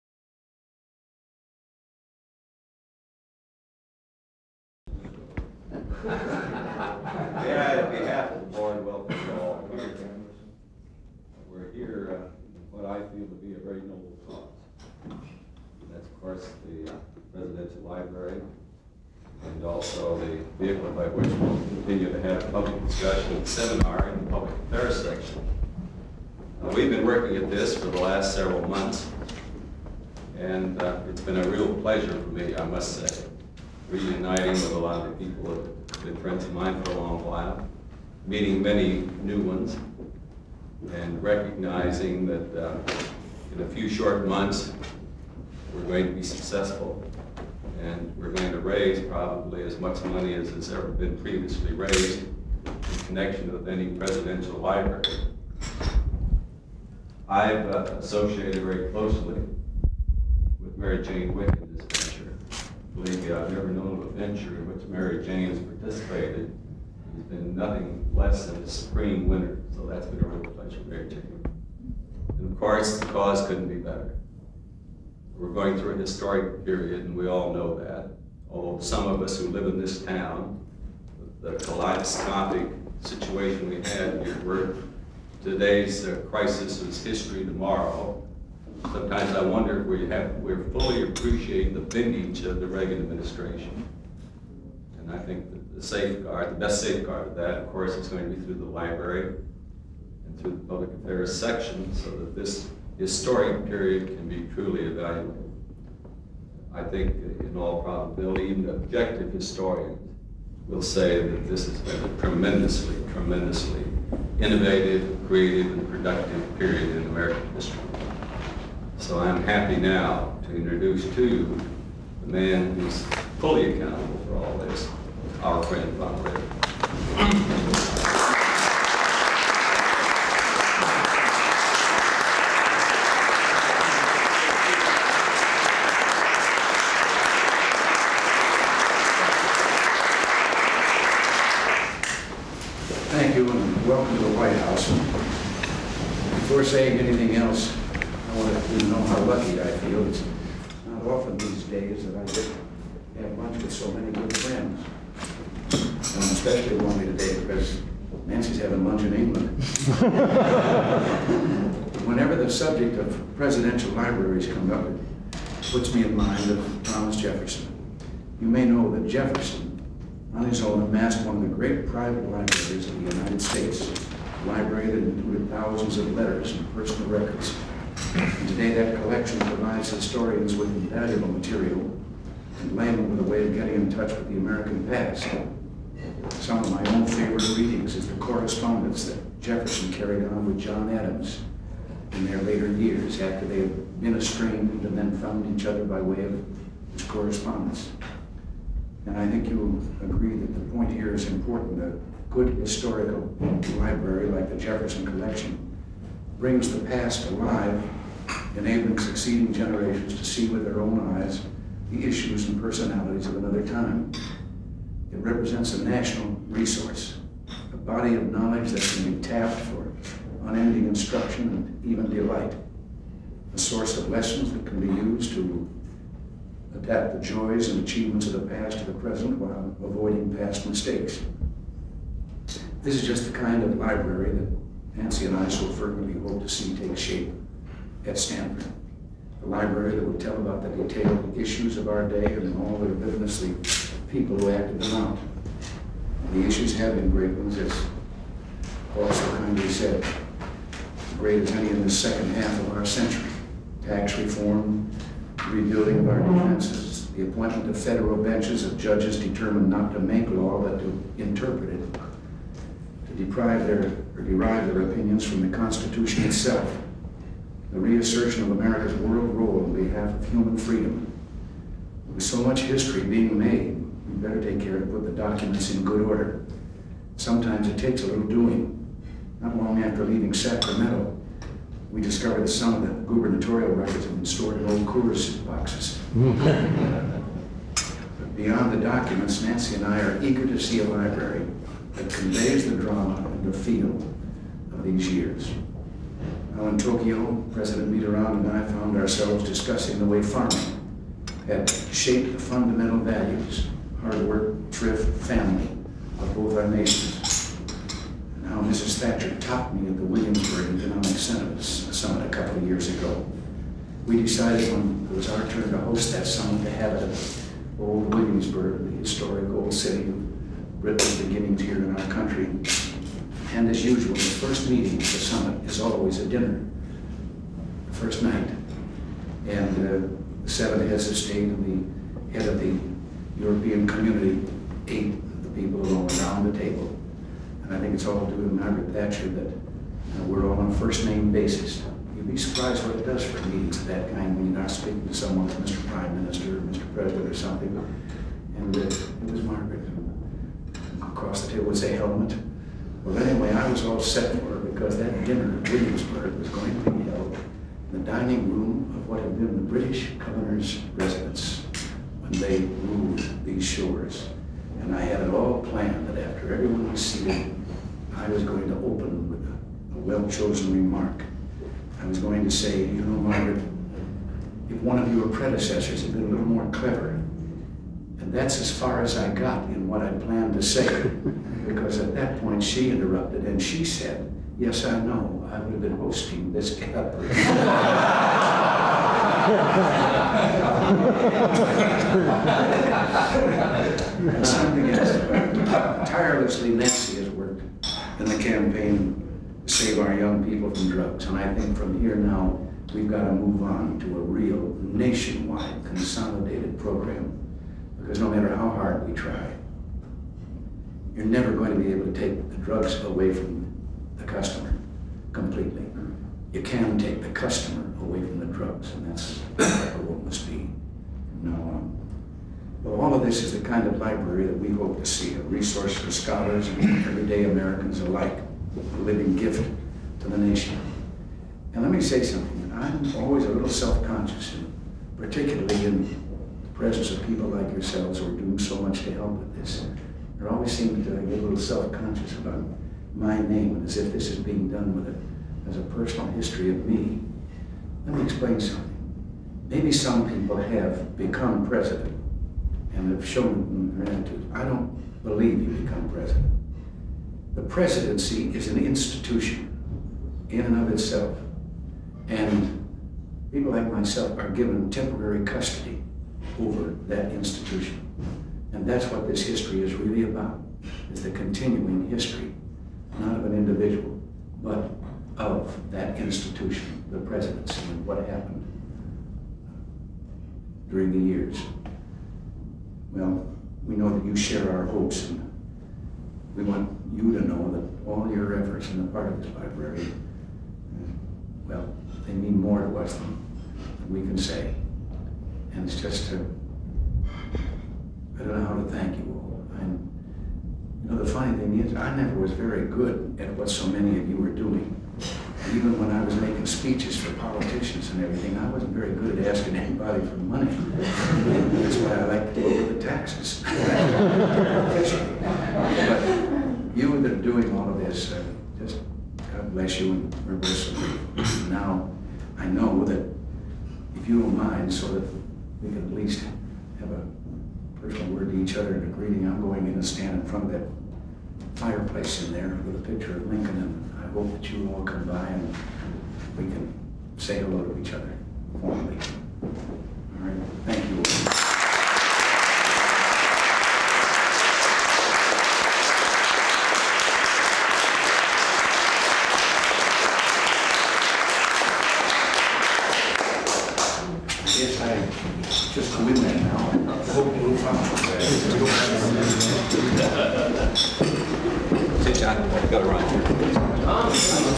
President Reagan's Remarks to Reagan Presidential Foundation on July 21, 1986
Full Title: Reagan Presidential Foundation Luncheon. President Reagan Attends Luncheon (Senator Paul Laxalt's Remarks) in Family Dining Room on July 21, 1986

Transcript: N/A

Production Date: 7/21/1986

Access Restriction(s):Unrestricted
Use Restriction(s):Unrestricted